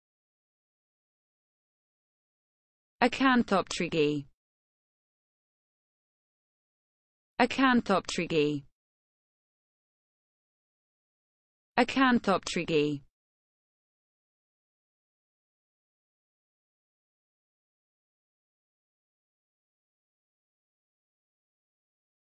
acanthopterygii: How to pronounce acanthopterygii with Phonetic and Examples
This video shows how to pronounce acanthopterygii correctly with phonetic and examples on how to use it
acanthopterygii Definition : teleost fishes having fins with sharp bony rays
acanthopterygii Phonetic : [acanthopterygii]